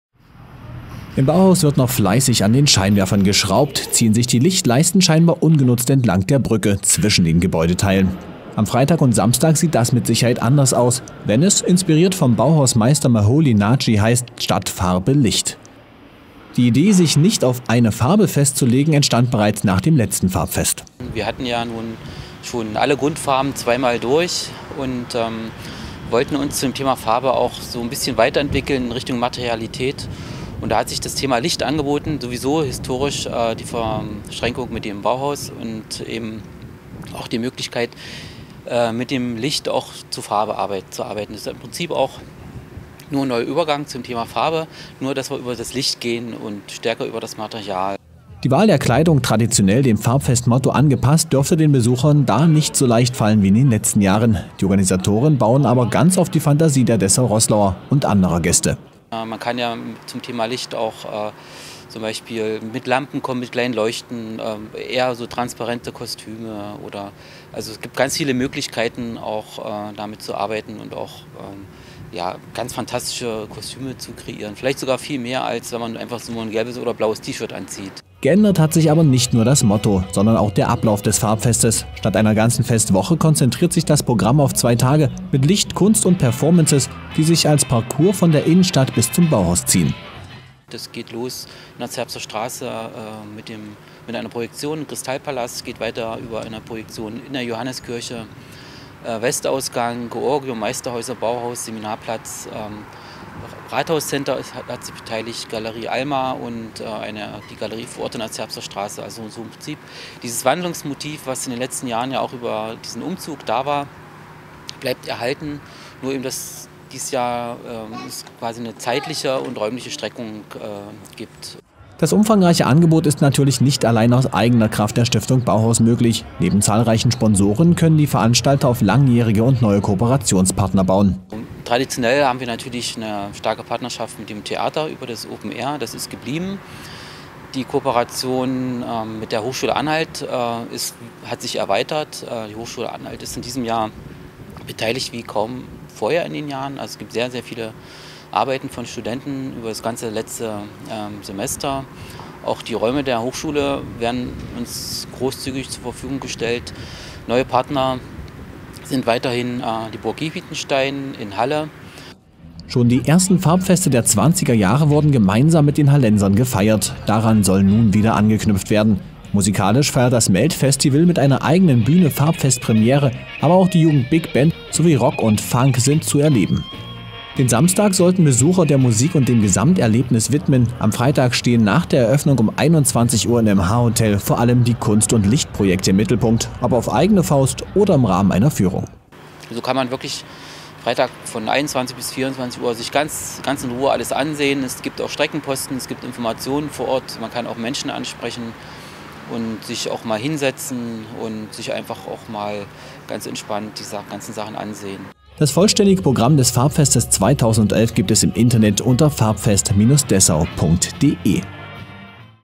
14. Dessauer Farbfest thematisiert Licht - RAN1
Seit 1997 wird in Dessau das Farbfest gefeiert, dessen Wurzeln in den historischen Bauhaus-Festen der 20er Jahre liegen. Nach 13 Ausgaben, die im Zeichen jährlich wechselnder Farben standen, heißt es diesmal "Statt Farbe: Licht". Aber eines ist sicher: farblos wird auch das 14. Farbfest ganz bestimmt nicht.